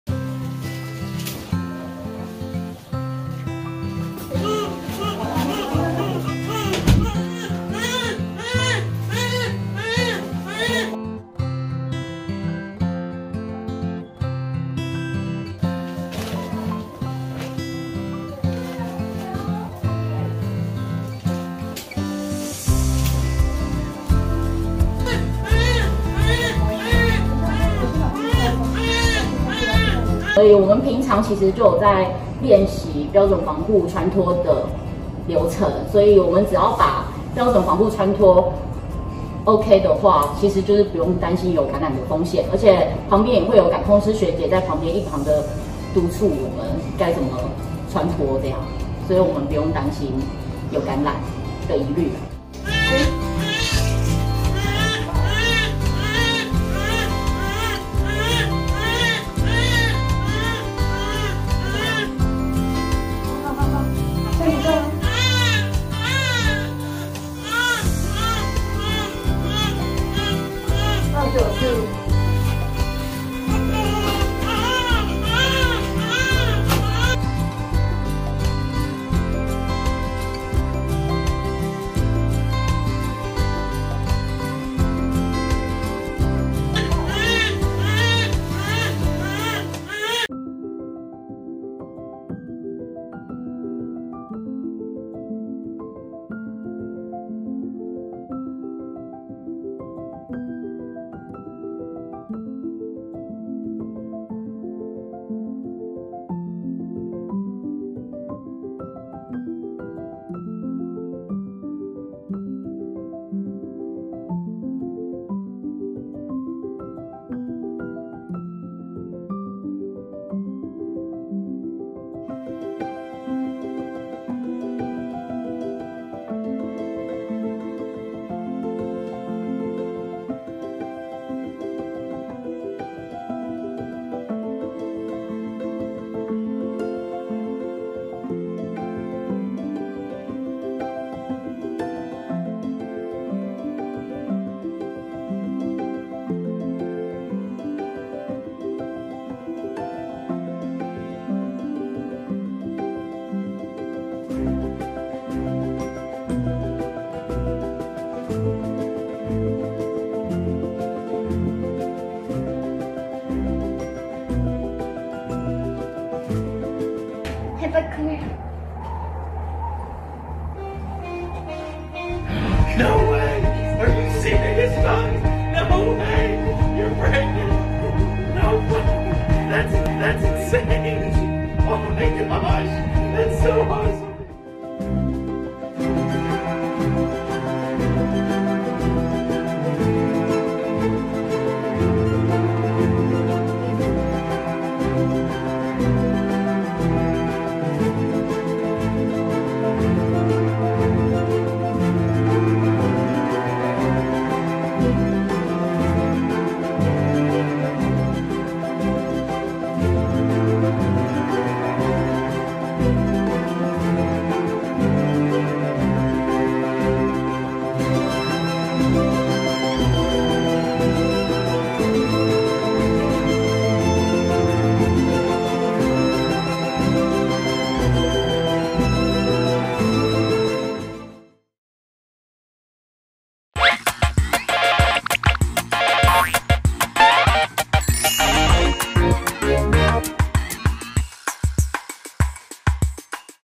確診婦剖腹產子「母乳有抗體」 產後分離...堅持擠奶護寶寶｜新冠肺炎｜林口長庚醫院｜新生兒
☑影片授權：林口長庚 提供